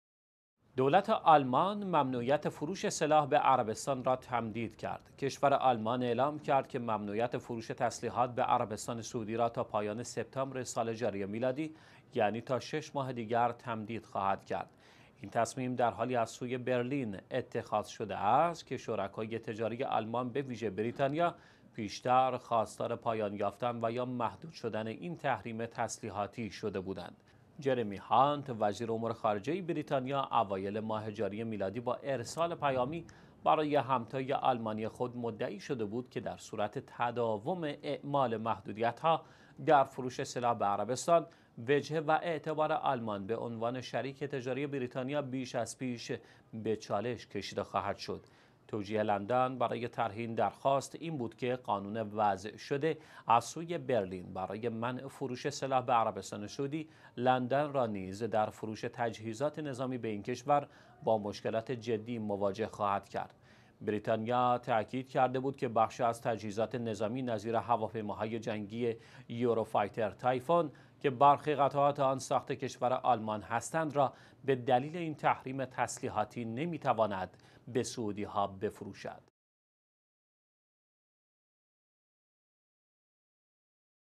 تمدید ممنوعیت فروش سلاح به عربستان، از سوی آلمان
دولت آلمان ممنوعیت فروش سلاح به عربستان را تمدید کرد.
کشور آلمان اعلام کرد که ممنوعیت فروش تسلیحات به عربستان سعودی را تا پایان سپتامبر سال جاری میلادی - یعنی تا شش ماه دیگر - تمدید خواهد کرد.
این تصمیم در حالی از سوی برلین اتخاذ شده است که شرکای تجاری آلمان به ویژه بریتانیا، پیش تر خواستار پایان یافتن و یا محدود شدن این تحریم تسلیحاتی شده بودند.
«جرمی هانت» وزیر امور خارجه بریتانیا اوایل ماه جاری میلادی با ارسال پیامی برای همتای آلمانی خود مدعی شده بود که در صورت تداوم اعمال محدودیت ها در فروش سلاح به عربستان، وجهه و اعتبار آلمان به عنوان شریک تجاری بریتانیا بیش از پیش به چالش کشیده خواهد شد.
توجیه لندن برای طرح این درخواست، این بود که قانون وضع شده از سوی برلین برای منع فروش سلاح به عربستان سعودی، لندن را نیز در فروش تجهیزات نظامی به این کشور با مشکلات جدی مواجه خواهد کرد.

بریتانیا تاکید کرده بود که بخشی از تجهیزات نظامی نظیر هواپیماهای جنگی «یوروفایتر تایفون»، که برخی قطعات آن ساخت کشور آلمان هستند را  به دلیل این تحریم تسلیحاتی،  نمی تواند به سعودی ها بفروشد.